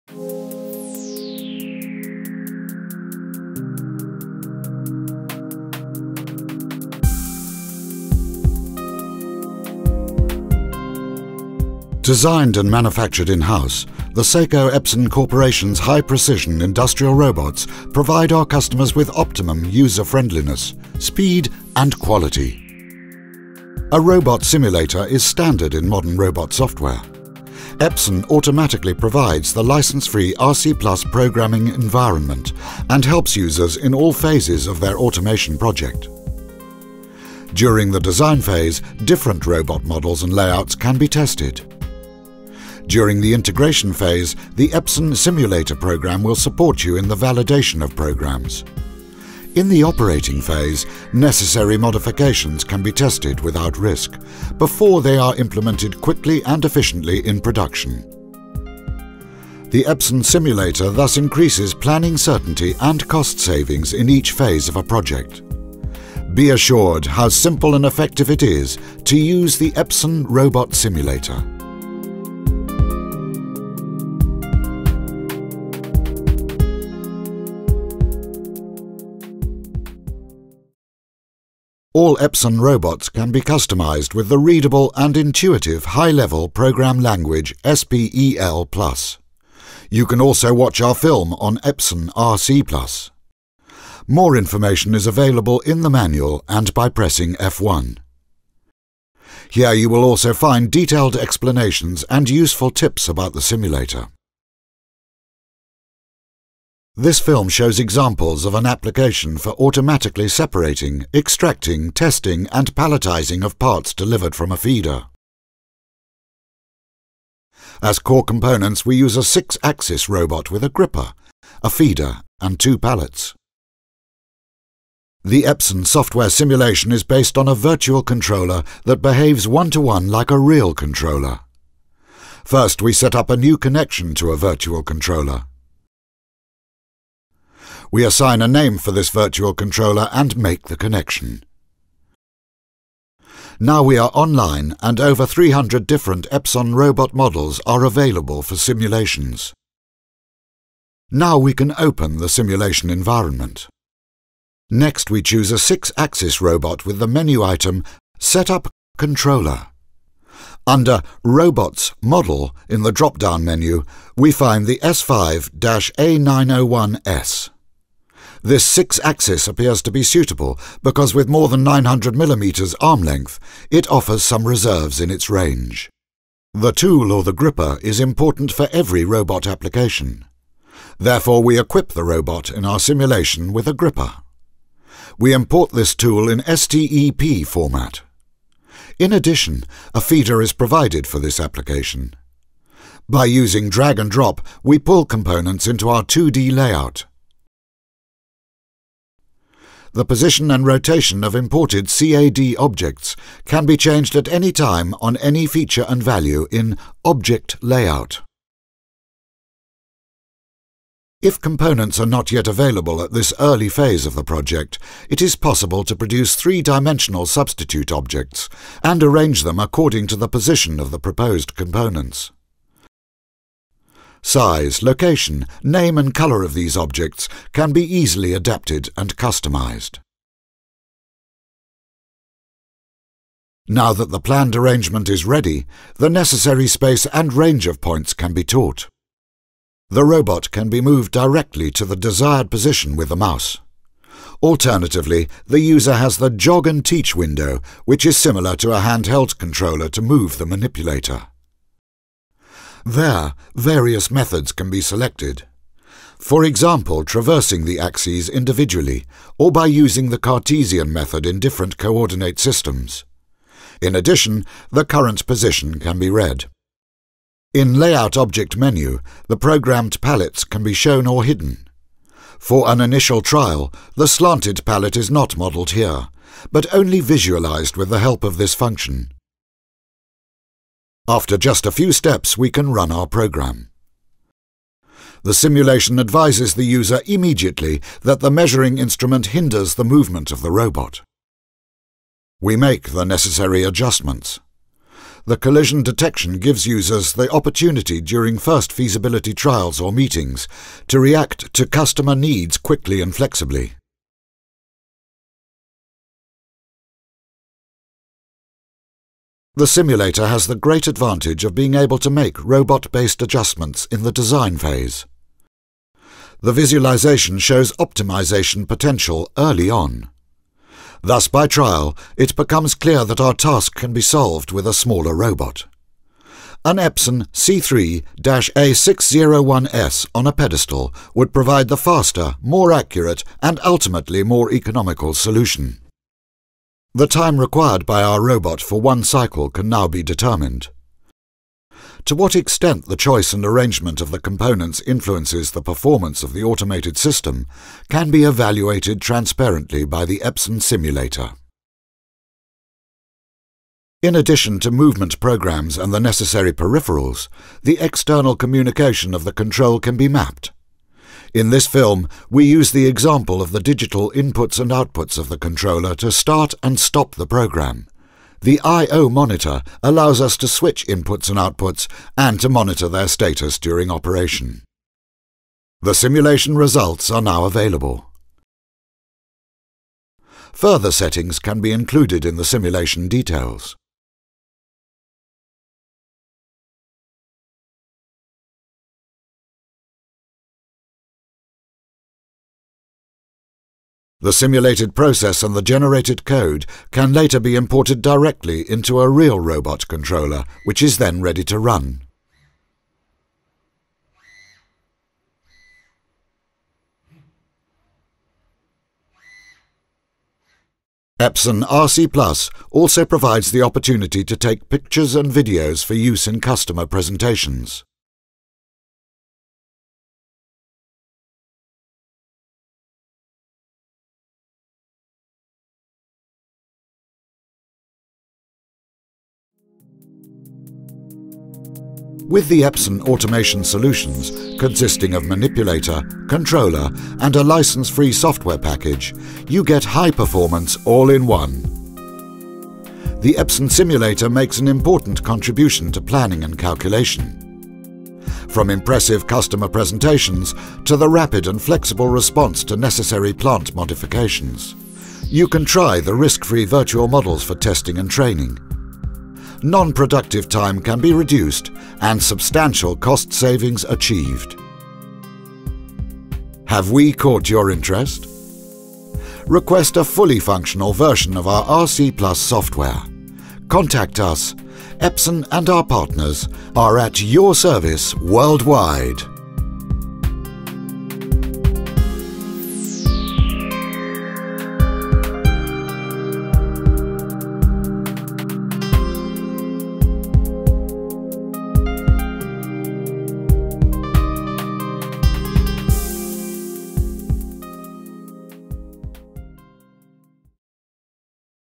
EPSON RC+ 5.0 Robot Simulator Software
EPSON RC+ 5.0 Robot Simulator Software makes it possible for automation engineers to design, build and debug virtual robot work cells - saving valuable time and money.

Here are some of the things users can do with EPSON RC+ 5.0 Simulator Software:
Display 3D Robot Motion
• Shows robot orientation and motion in a 3D display from various viewpoints.
• Offers accurate display data based on design data.

Check of Mechanical Interference Problems
• Checks whether the robot (including the end of arm tooling) interferes with itself or its peripherals.

Estimate Robot Cycle Times
• Predicts the robot operation time for a program.
• Considers the speed setting (Speed, etc.) and acceleration / deceleration setting (Accel, etc.) when predicting the robot motion time.

Execute SPEL+ programs
• Allows you to create, execute, and debug SPEL+ programs.